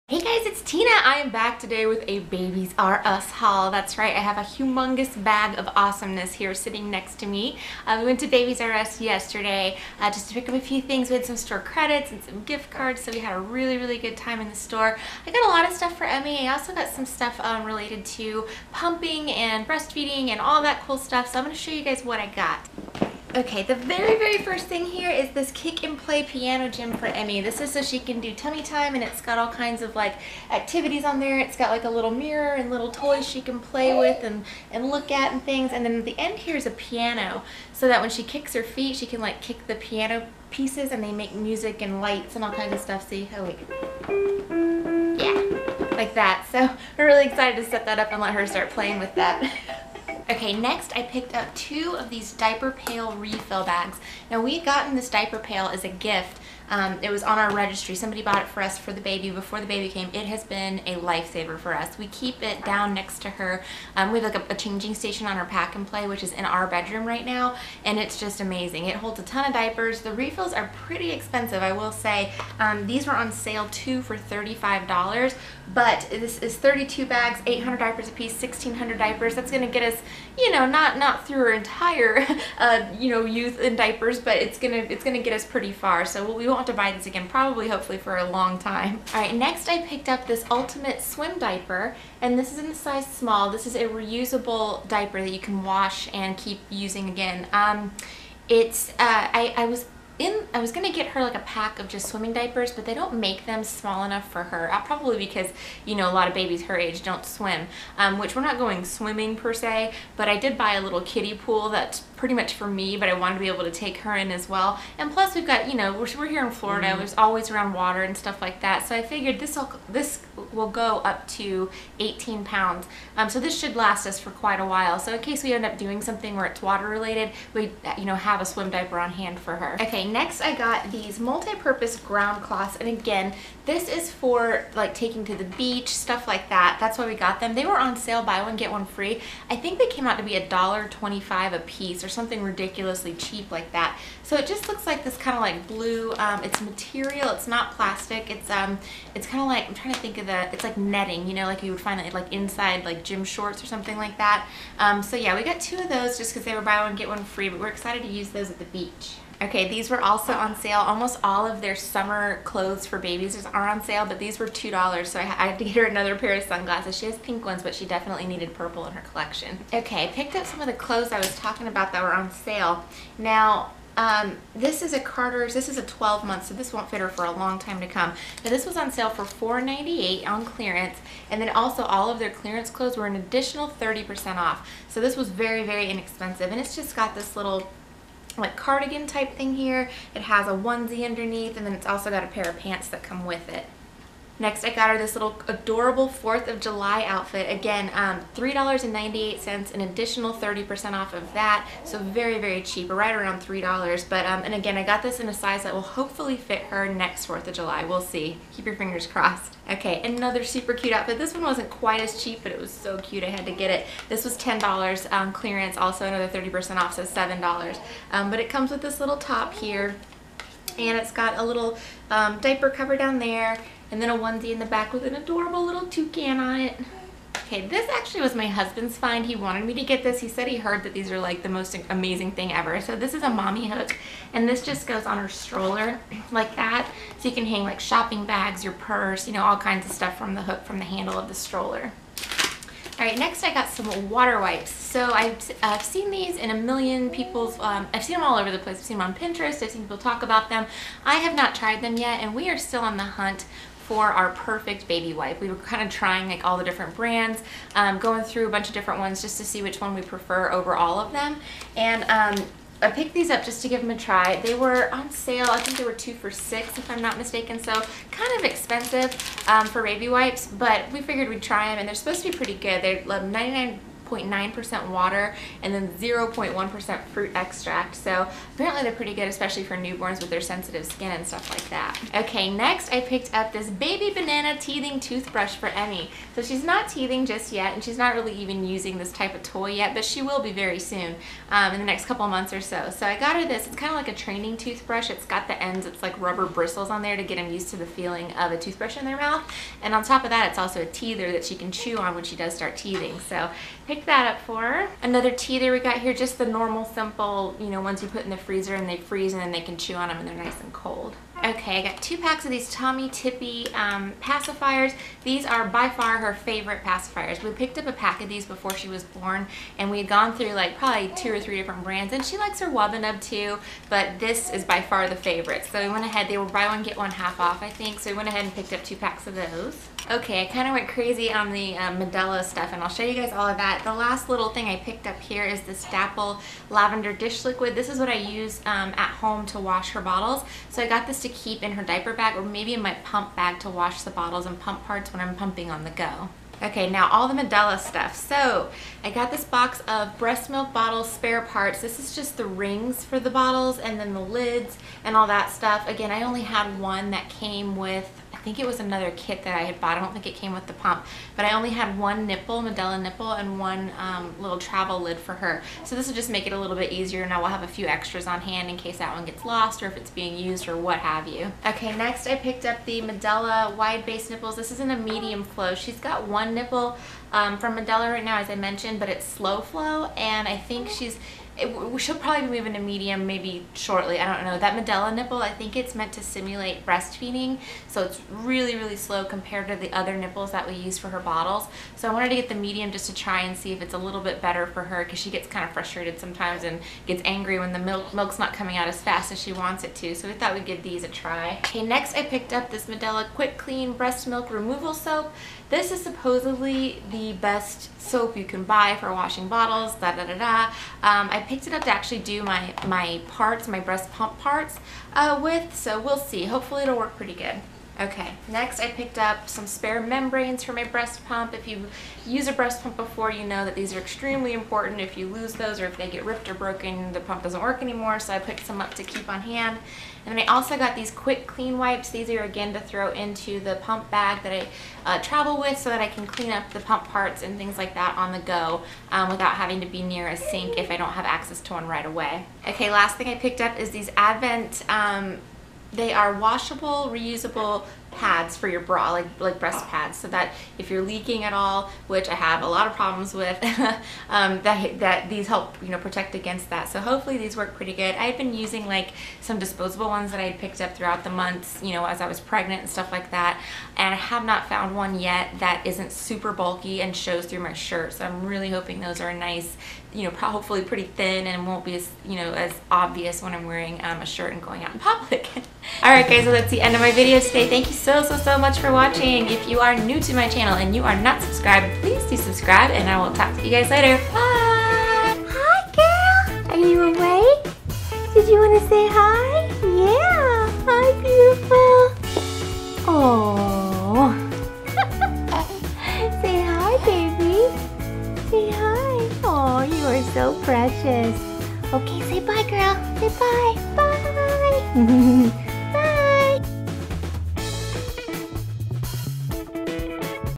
Babies R Us Haul! Clearance Clothes, Toys, & Pumping Supplies + Emmy Cameo!
Ebates- Earn $$ for shopping online!